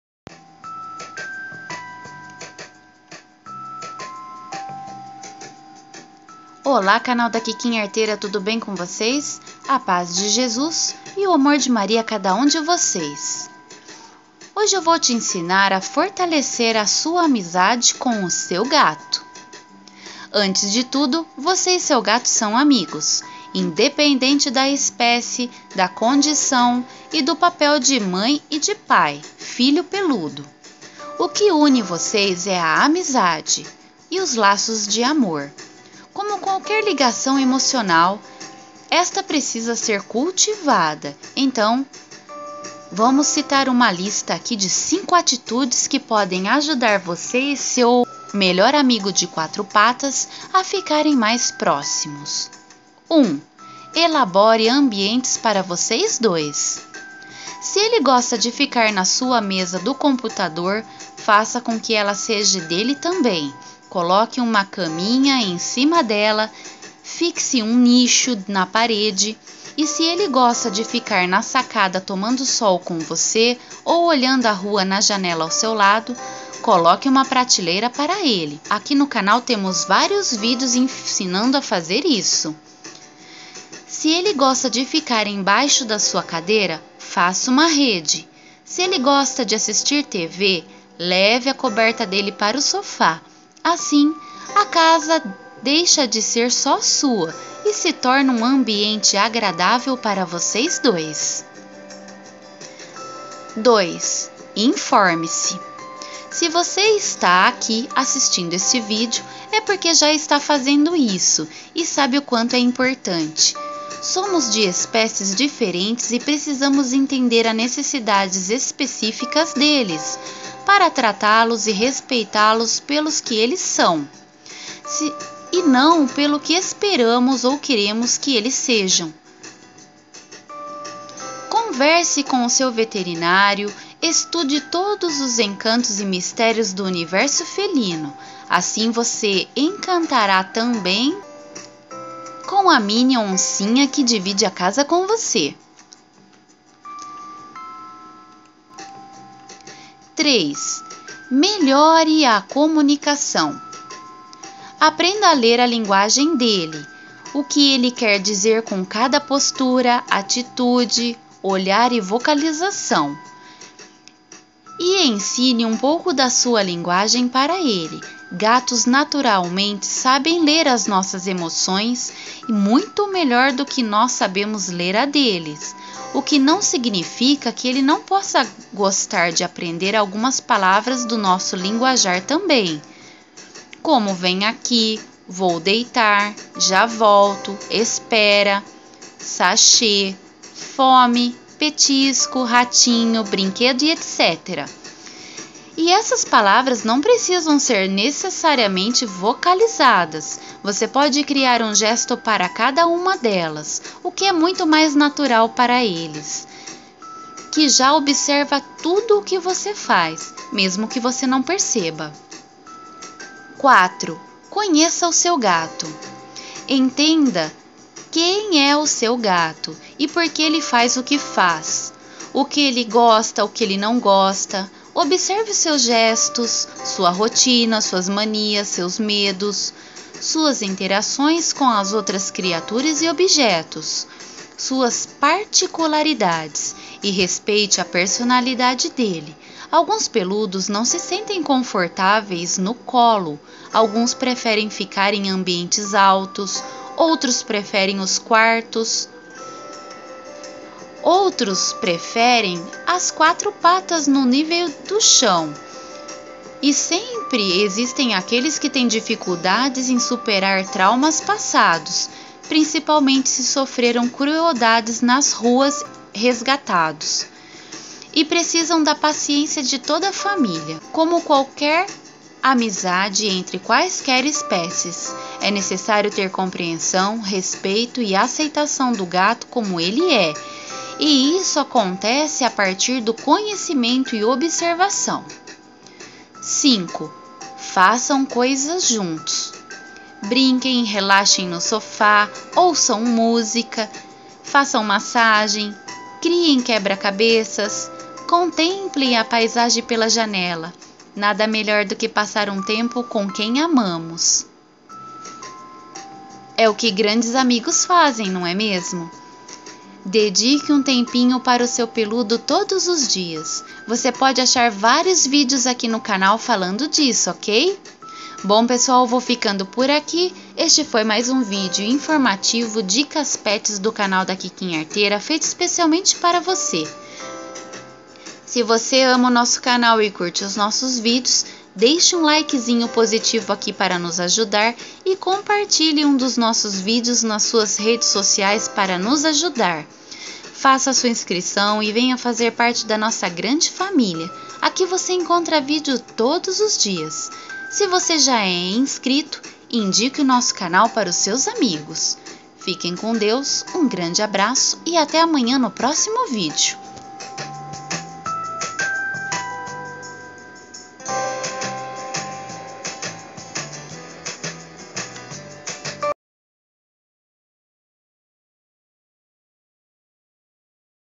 Fortaleça a amizade com o seu gato
A comunicação é a chave para uma amizade pura e verdadeira. Dito isso, venha comigo e conheça melhor seus gatos e fortaleça a amizade.